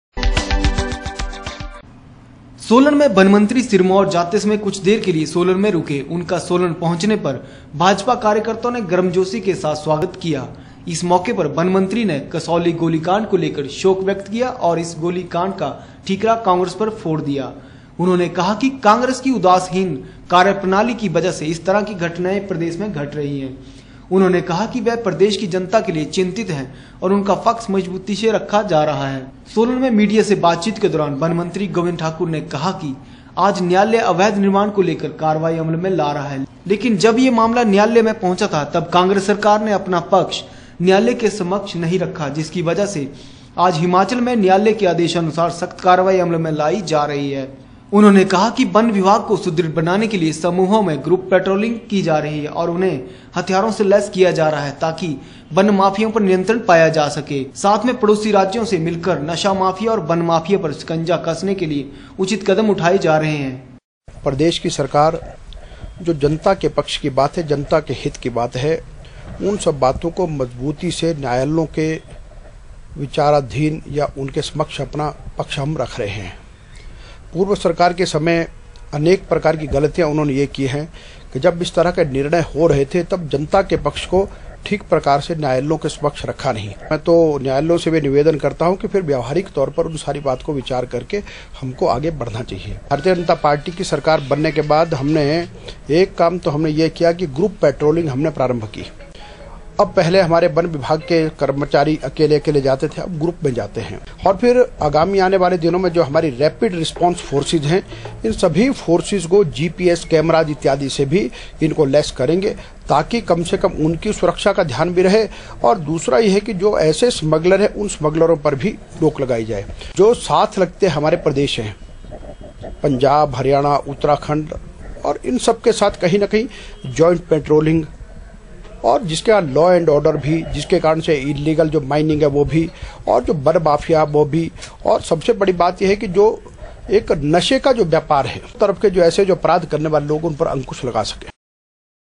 सोलन में वन मंत्री सिरमौर जाते समय कुछ देर के लिए सोलन में रुके
सोलन में वन मंत्री सिरमौर जाते समय कुछ देर के लिए सोलन में रुके | उनका सोलन पहुंचने पर  भाजपा कार्यकर्ताओं ने गर्म जोशी के साथ स्वागत किया | इस मौके पर वन मंत्री ने कसौली गोली कांड को लेकर शोक व्यक्त किया और इस गोली कांड का ठीकरा कांग्रेस पर फोड़ दिया | उन्होंने कहा कि कांग्रेस की उदासहीन कार्यप्रणाली की वजह से इस तरह की घटनाएं प्रदेश में घट रहे है | उन्होंने कहा कि वह प्रदेश की जनता के लिए चिंतित है और उनका पक्ष मजबूती से रखा जा रहा है |